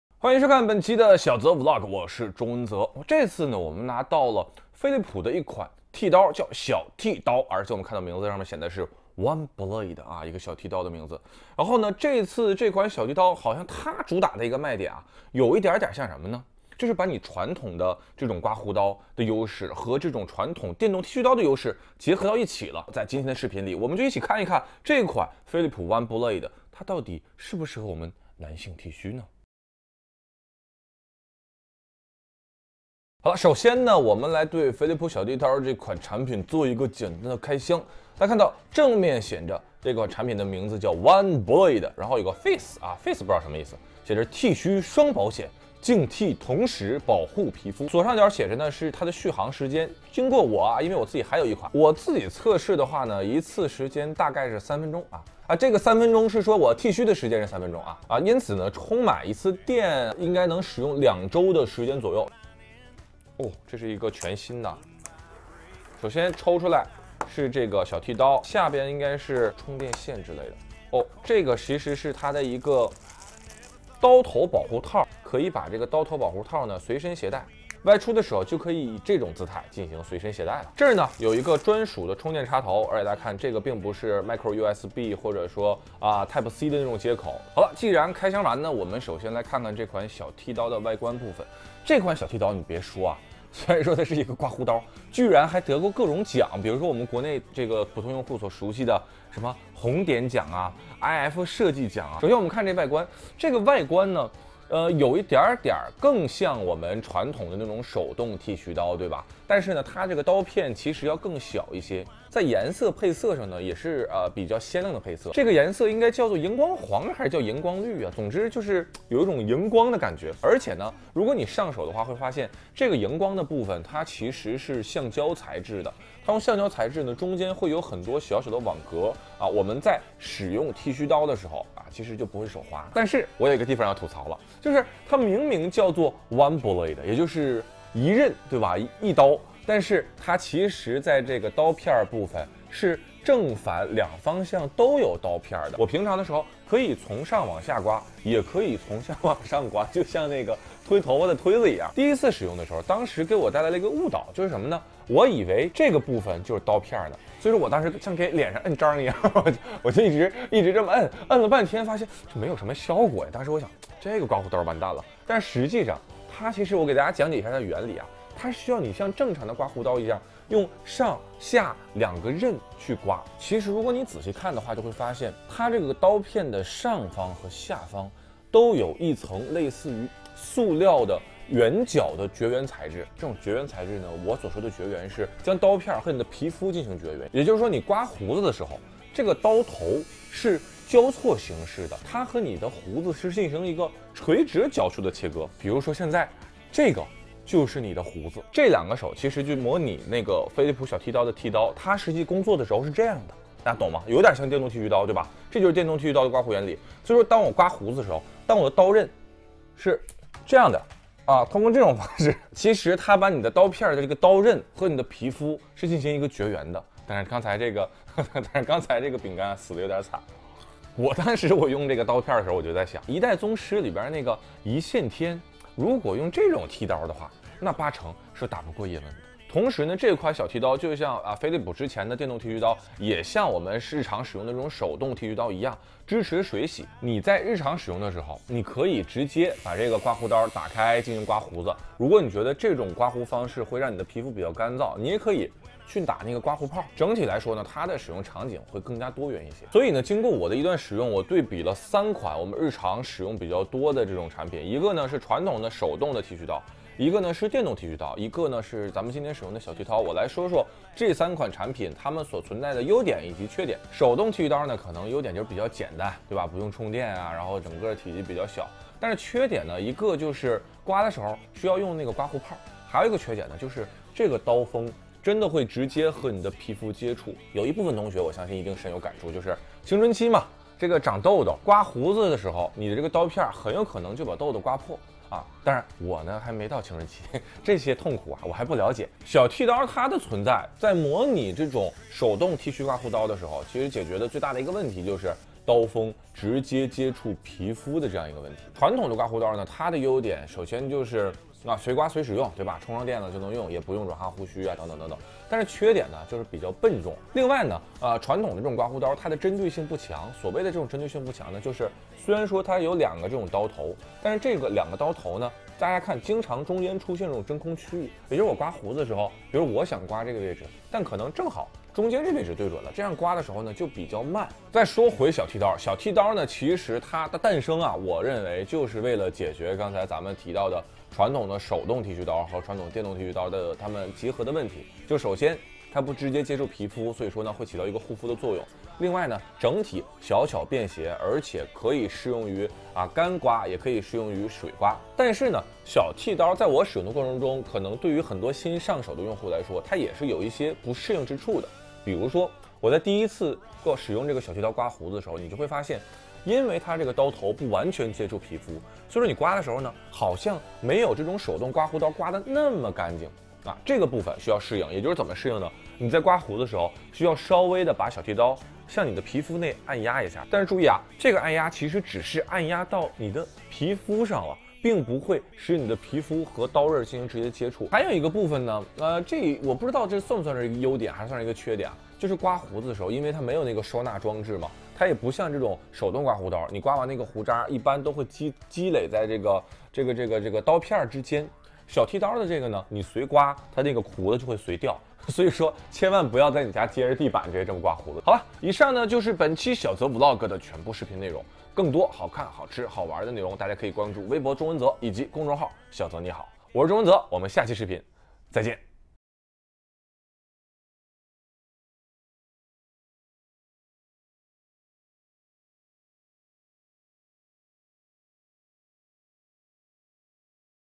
小泽VLOG：飞利浦小T刀评测 这到底是手动剃须刀还是电动剃须刀？
用了两三个月，应该是最慢的一期评测了，不过体验的比较完整。微博：钟文泽